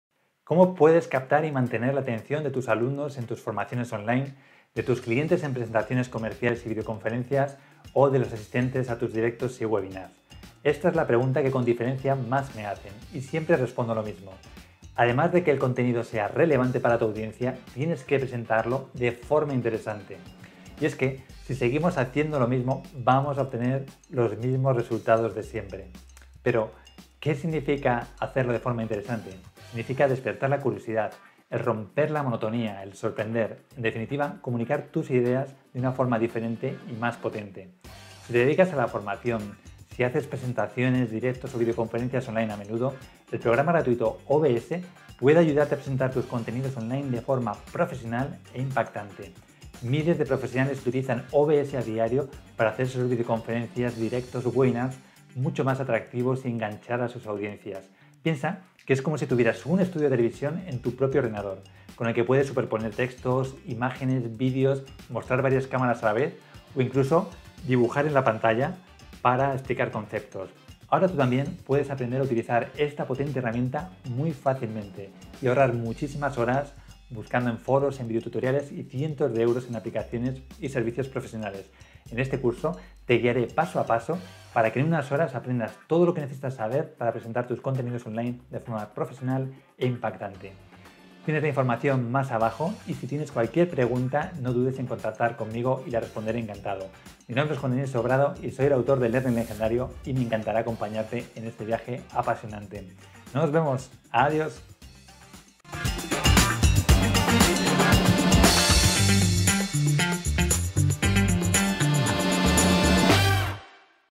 Nuevo Curso Impacta y Conecta Online: OBS Masters of Universe
Aprende a captar y mantener la atención de los participantes en videoconferencias, sesiones online, webinars e incluso videotutoriales.

Con este curso aprenderás a usar OBS y ahorrar cientos de euros en aplicaciones o servicios profesionales, y también cientos de horas, a la hora de grabar tus propios videos y contenidos formativos.